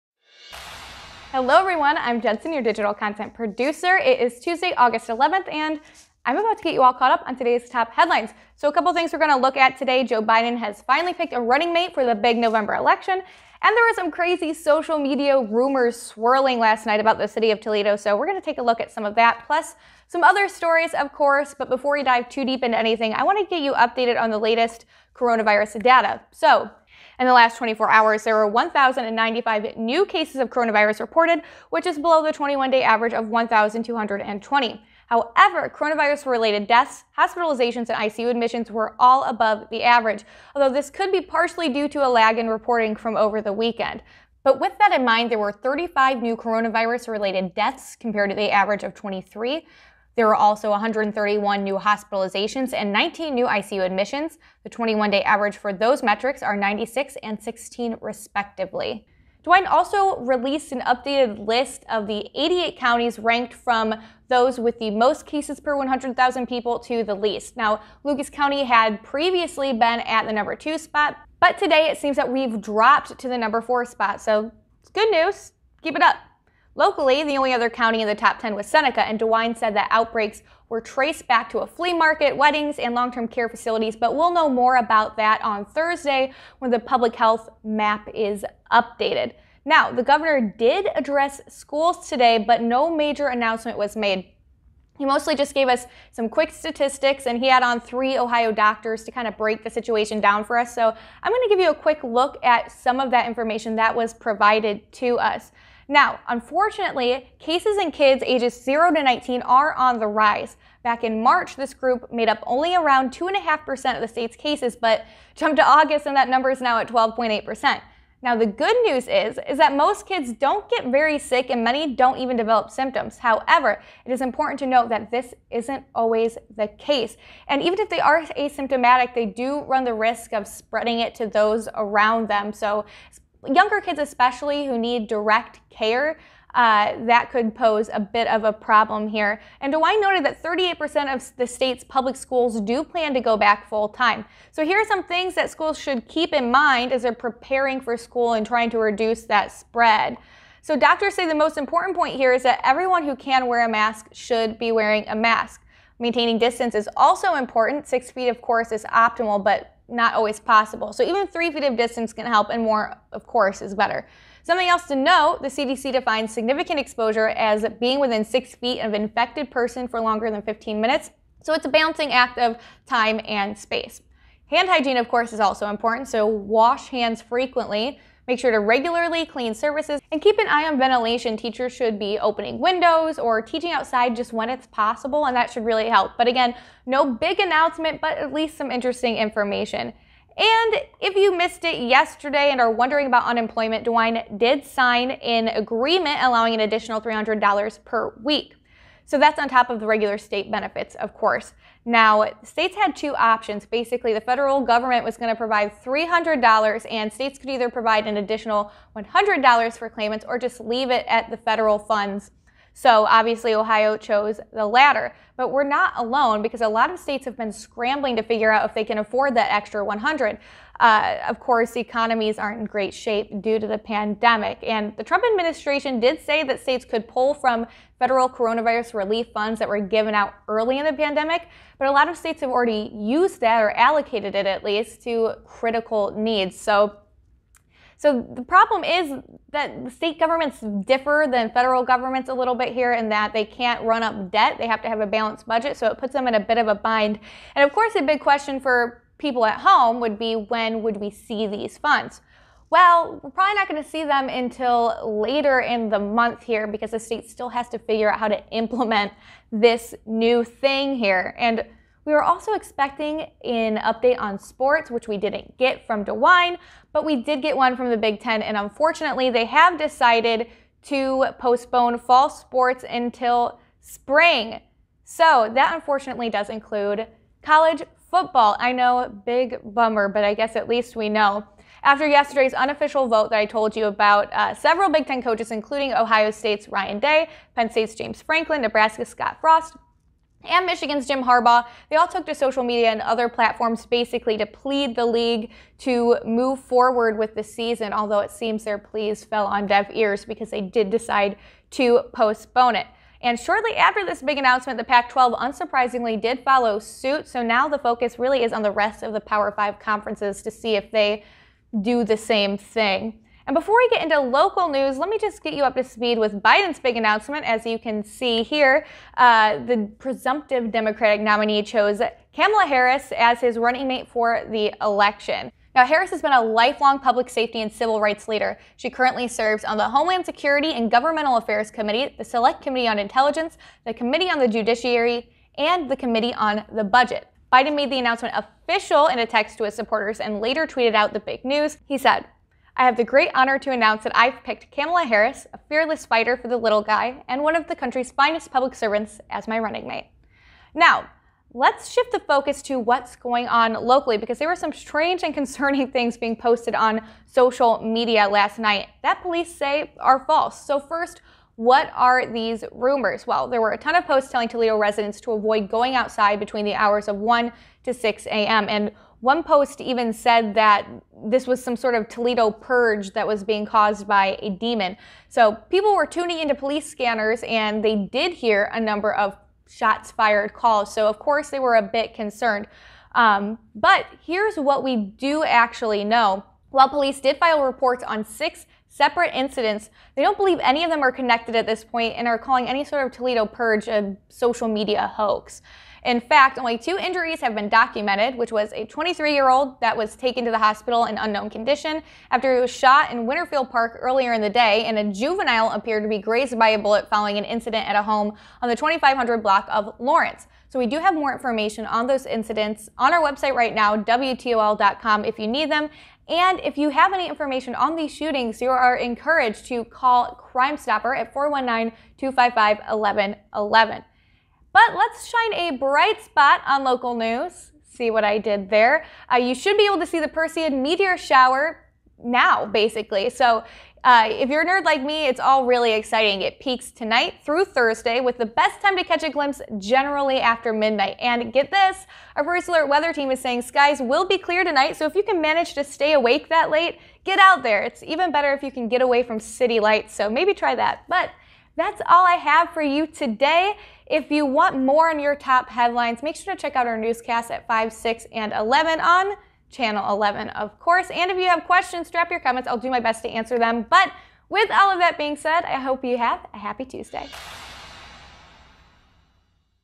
Joe Biden's VP pick, Ohio unemployment benefits | Northwest Ohio Top Headlines - Aug. 11, 2020
Jenson breaks down strange internet rumors in Toledo overnight, an update on Ohio unemployment and presumptive Democratic nominee Joe Biden's running mate pick.